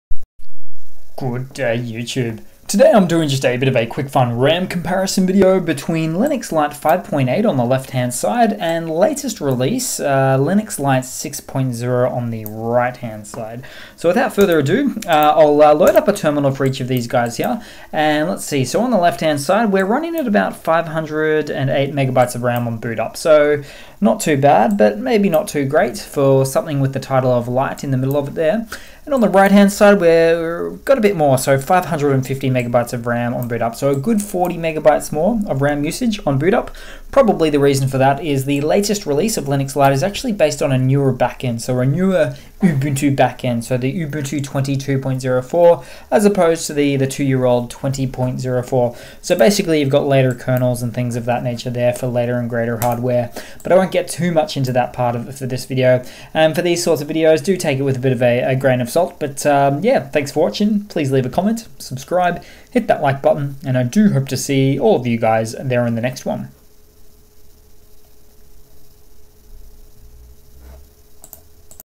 Linux Lite 5.8 vs Linux Lite 6: RAM Usage
Never give up on believing in the potential of Linux Lite to actually one day be 'Lite'. and not just 'Light'.